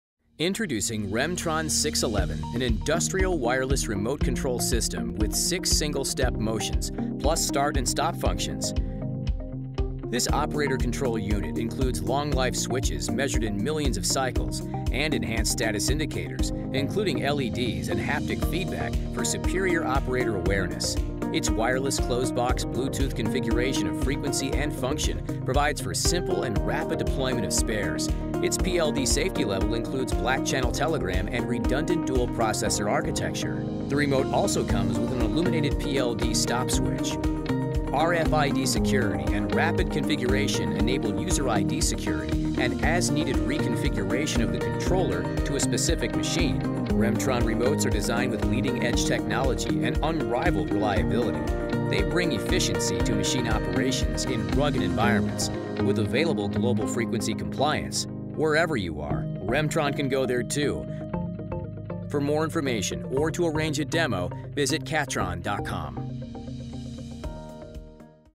Remtron™ 611 Industrial Wireless Remote Control Product Overview
Remtron™ 611 is a six single-step motions plus start and stop functions.  Remtron-brand products are used for the control of equipment and machinery in a wide variety of material handling, mobile equipment and industrial automation applications.